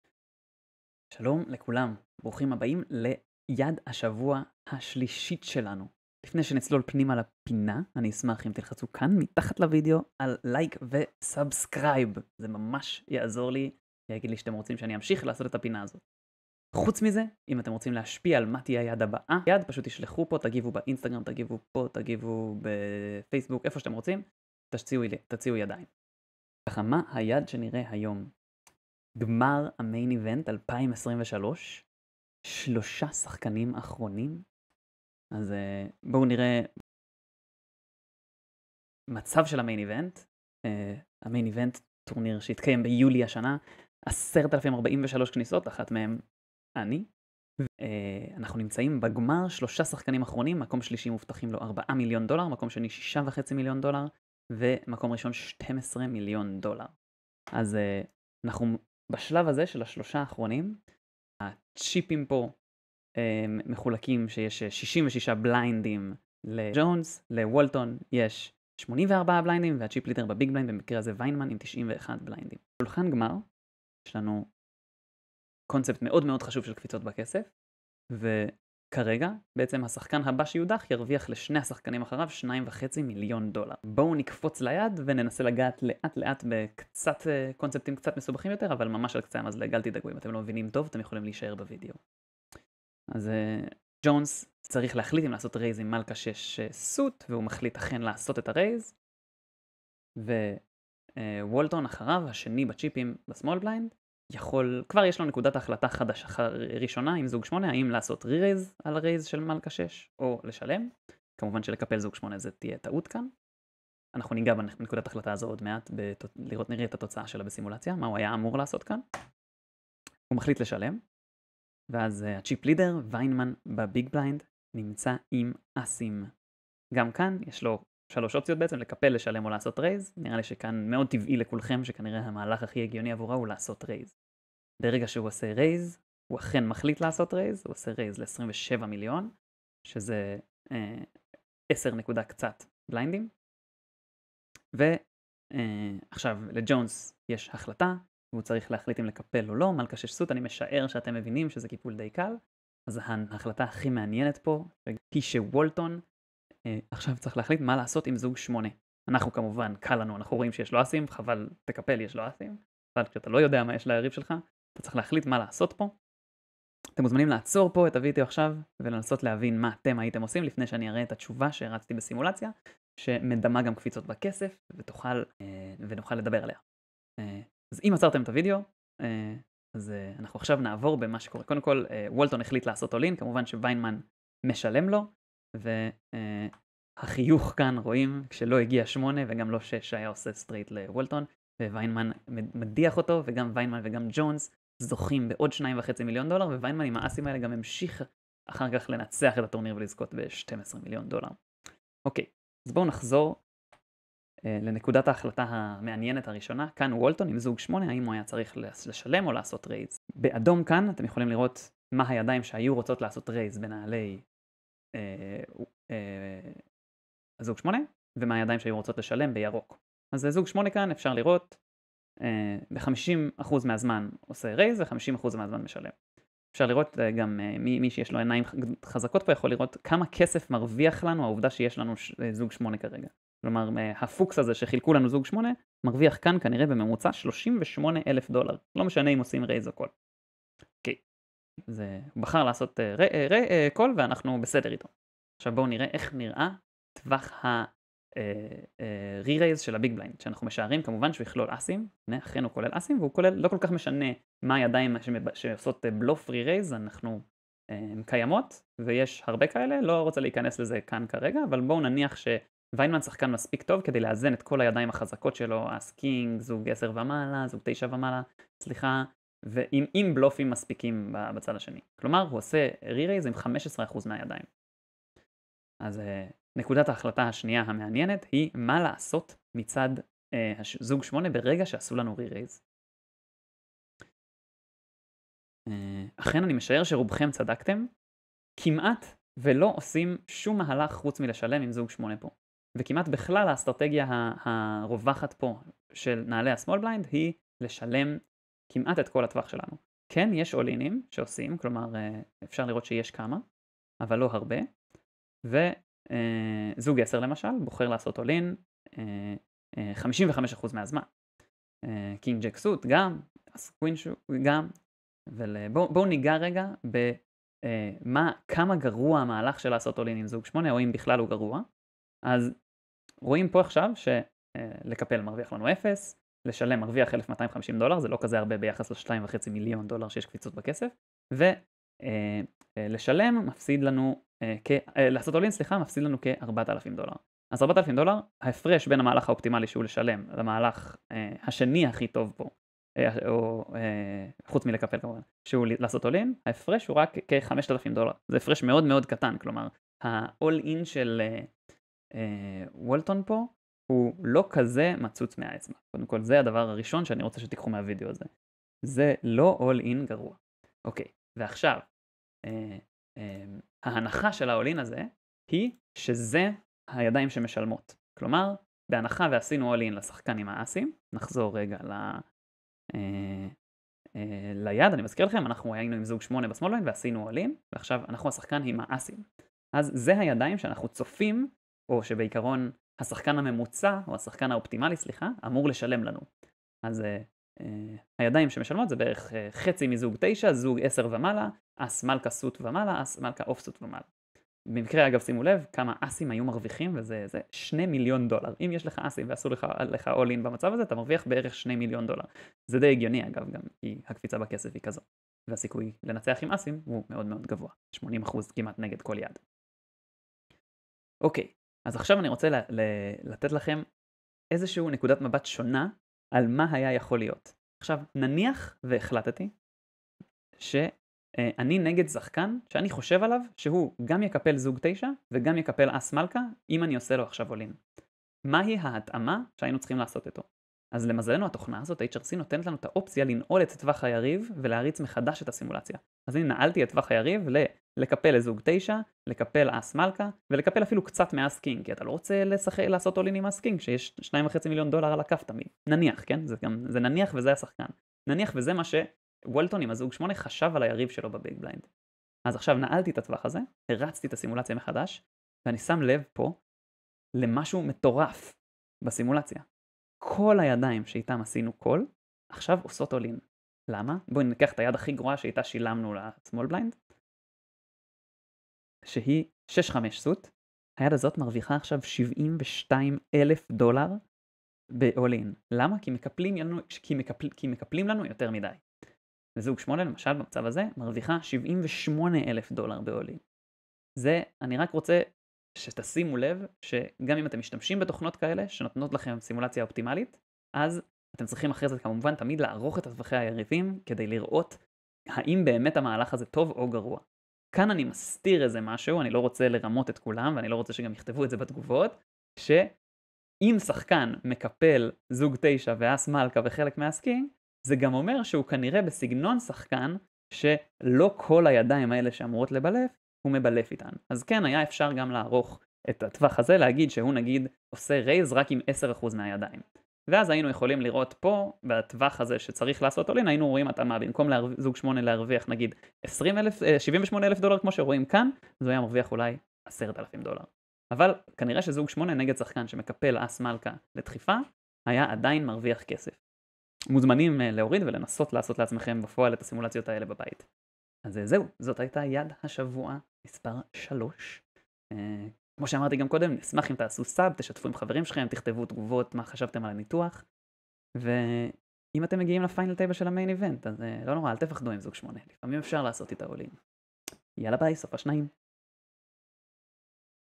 יד השבוע #3: הדחיפה שעלתה 2.5 מיליון דולר!
יד השבוע מספר 3!
לקחנו יד מגמר המיין איוונט 2023 וניתחנו האם באמת זוג 88 הייתה טעות ששווה מיליונים?